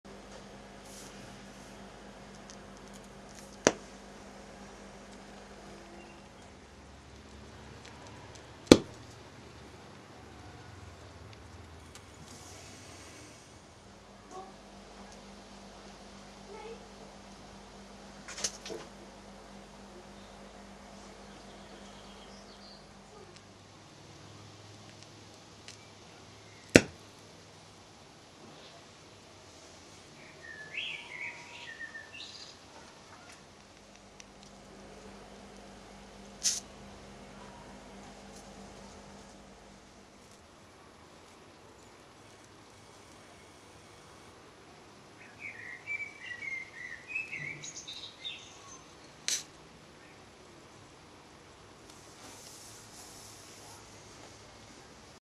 Chemistry experiment 7 - Purple smoke
Some pyrotechnix mixes are heated until they explode. Purple iodine vapor is formed.